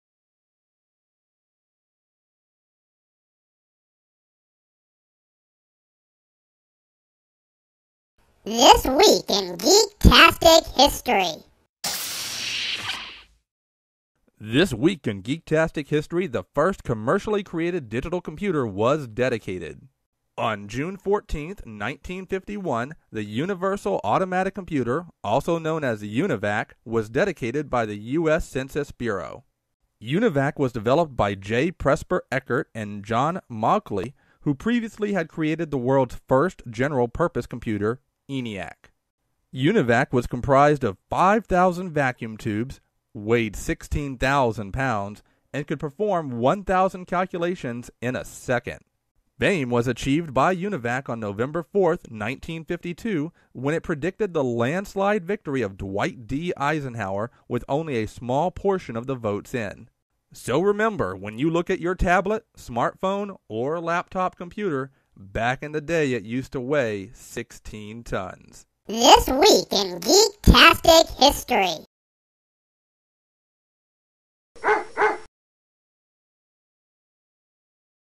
The Breaux Show - A Video Short - This Week in Geektastic History - June 10 to June 16
This Week in Geektastic History

This week on June 14, 1951, the first commercially available electronic digital computer was dedicated by the US Census Bureau.

UNIVAC, the follow-up computer to ENIAC, was put to use by the US Census Bureau and later by the election committee of the US to correctly predict the landslide victory of Dwight D. Eisenhower in the Presidential Election that same year.

UNIVAC was created by J. Presper Eckert and John Mauchly, and consisted of 5000 vacuum tubes, and weighed 16,000 pounds.

You can also follow my wackiness almost daily on my twitter

Come back for more fun and wackiness!